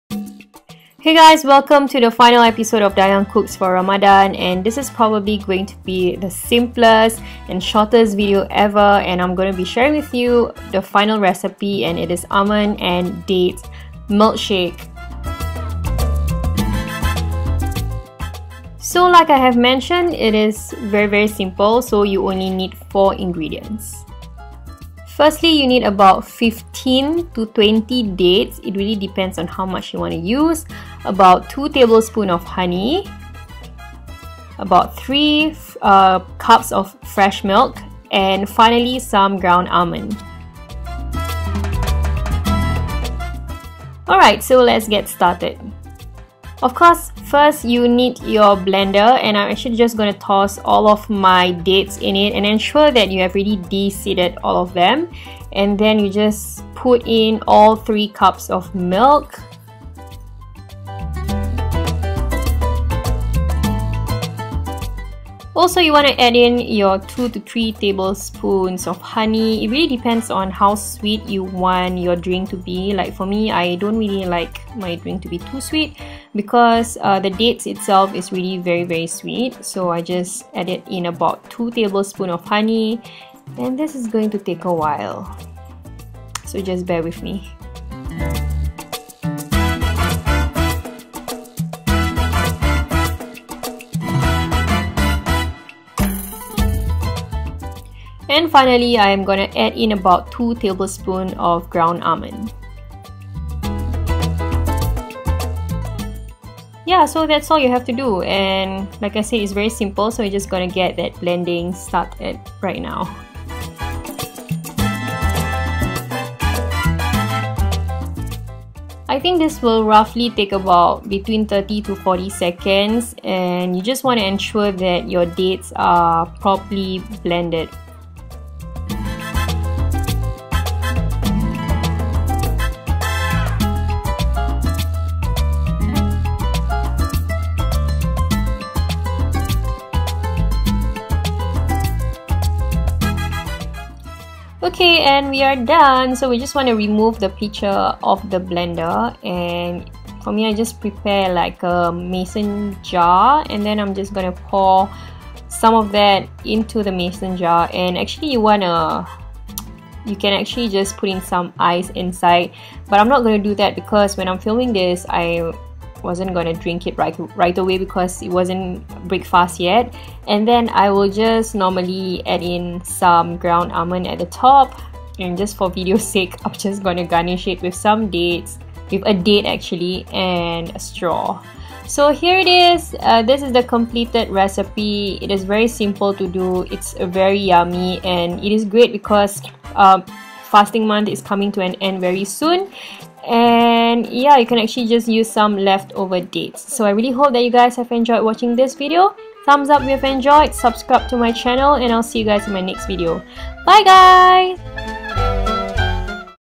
DayangCooks for Ramadan | Almond and Dates Milkshake | dygans90
Watch in HD! More info listed below!

Ingredients:
- 15 pieces of dates
- 2 tablespoon of honey
- 3 cups of milk
- 2 tablespoon of ground almond

Tech details:
Camera: Samsung NX Mini: 9-27mm
Editing Software: iMovie '11
Lighting: Ring Light NG-65C

xo Dayang